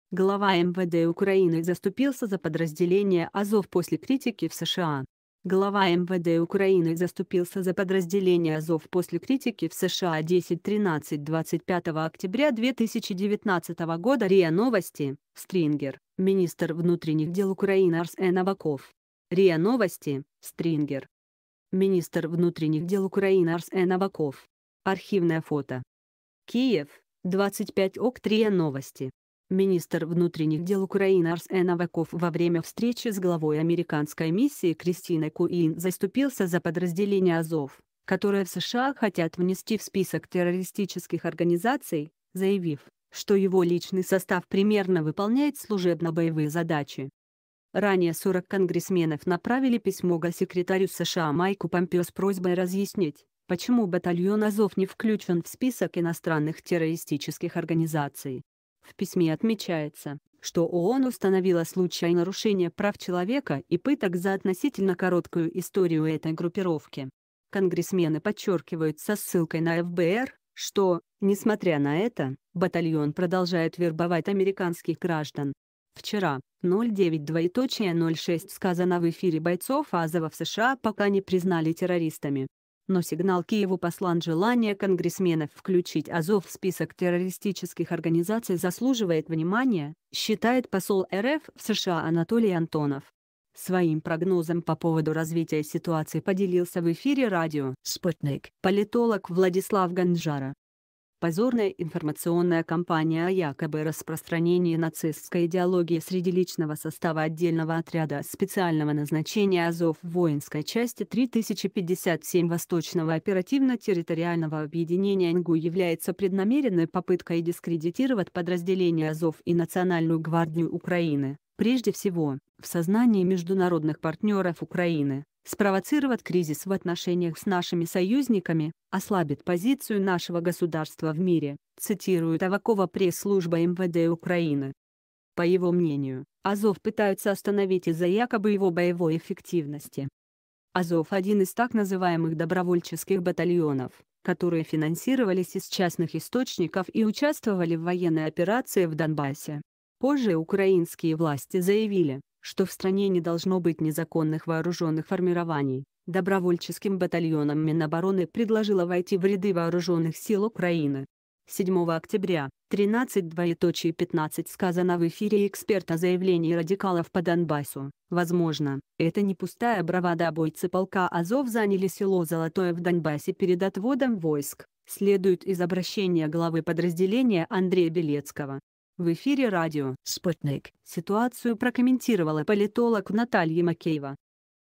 Глава МВД Украины заступился за подразделение Азов после критики в США
Глава МВД Украины заступился за подразделение Азов после критики в США Глава МВД Украины заступился за подразделение Азов после критики в США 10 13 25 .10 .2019 РИА Новости Стрингер Министр внутренних дел Украины Арсен Аваков РИА Новости Стрингер Министр внутренних дел Украины Арсен Аваков . Архивное фото КИЕВ, 25 окт - РИА Новости . Министр внутренних дел Украины Арсен Аваков во время встречи с главой американской миссии Кристиной Куиин заступился за подразделение Азов , которое в США